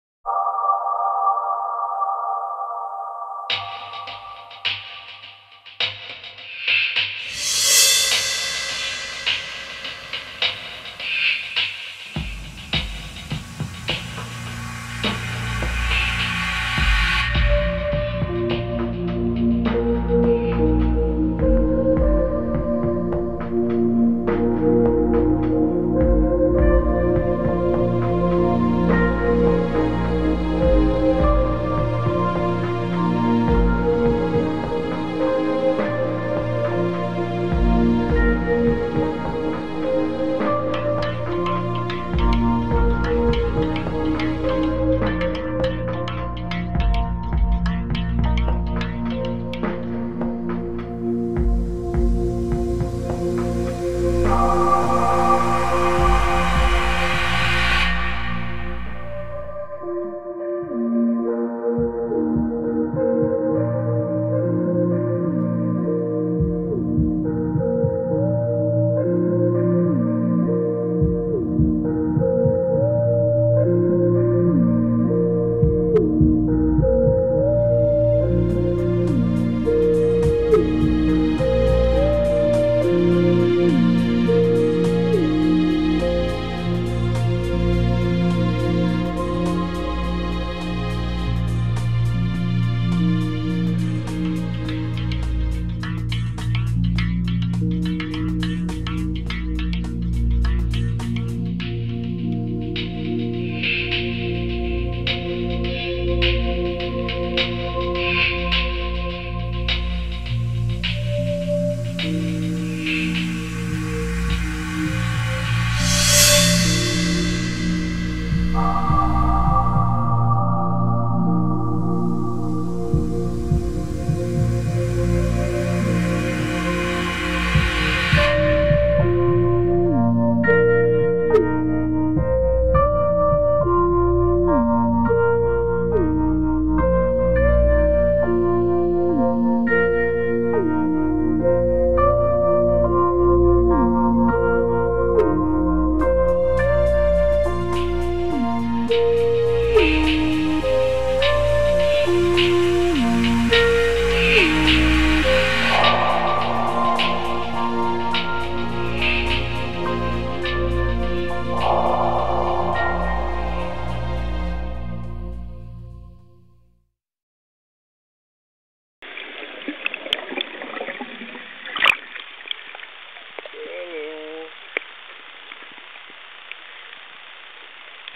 אימון צלילה חופשית משקולות משתנות 79 מטר ב 2:38 דקות
אימון צלילה חופשית במשקולות משתנות